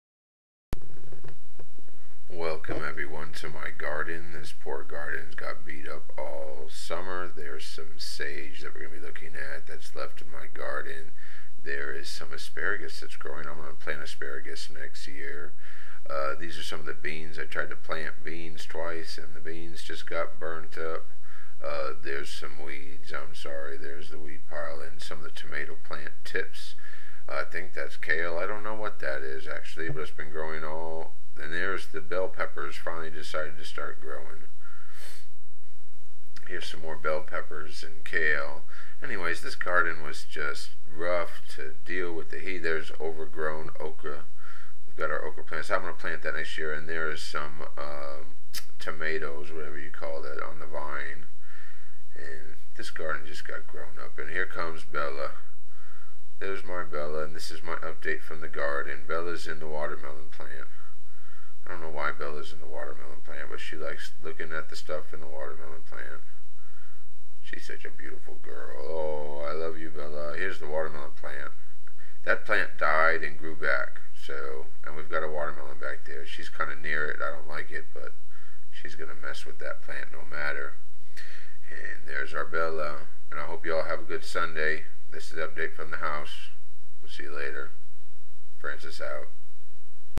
Update from the Hugelkultur Garden with Francis
This is what you find in our back yard. I hope it's worth the watch. Have a great Sunday. I will be setting the observatory pier pipe in the hole tomorrow! Go Texans! (It's a good game between the New Orleans Saints and the Houston Texans. I want the Texans to win of course.

Francis